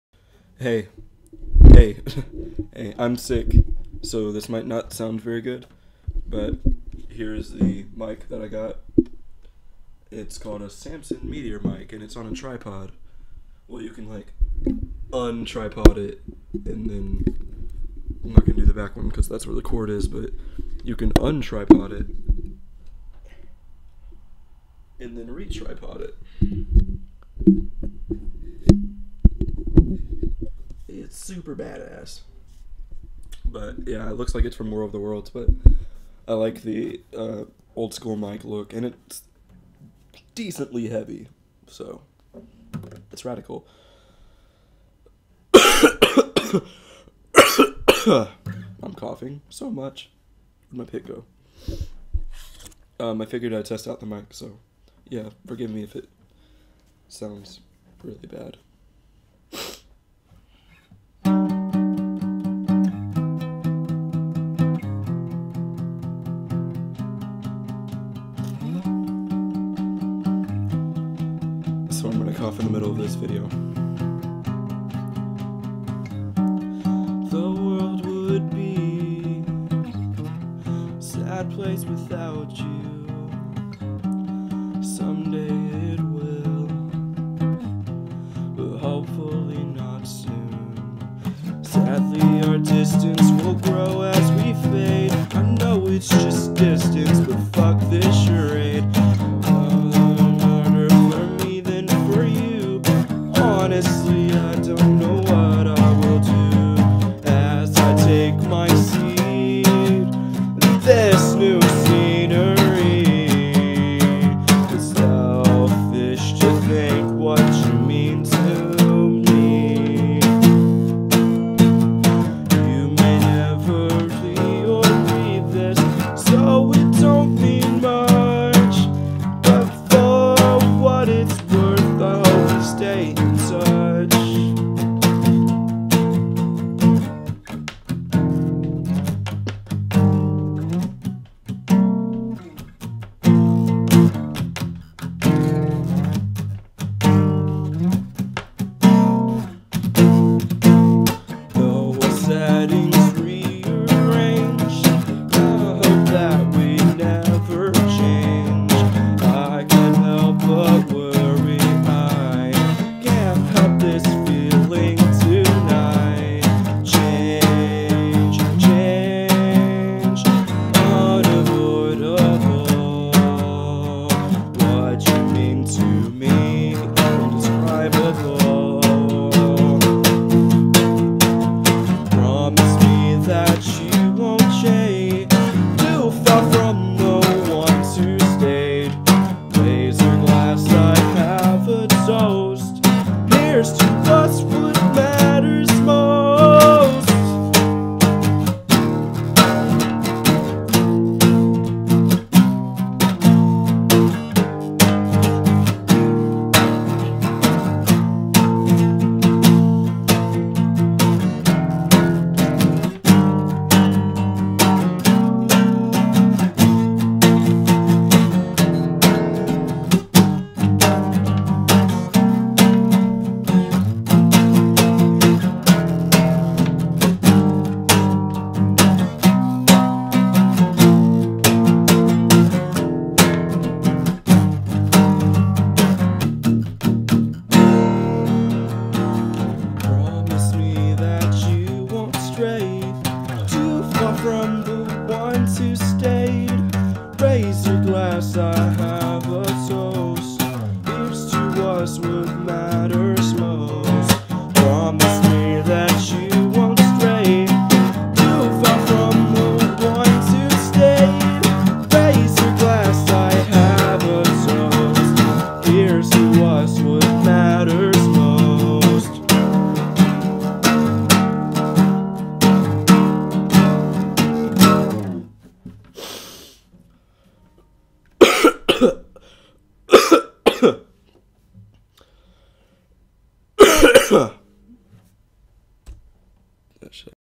Boy, does this sound like shit
I'm very coughy, but I'm testing this mic jones. Don't make fun of me.